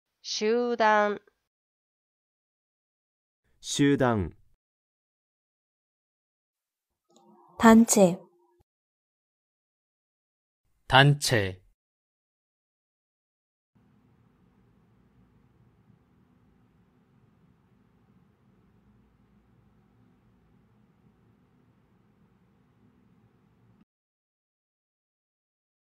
일본어로 "단체" 말하는 법
남자 목소리 :
集団

여성 목소리 :
集団

남성 음성 번역 :
단체

여성 음성 번역 :
단체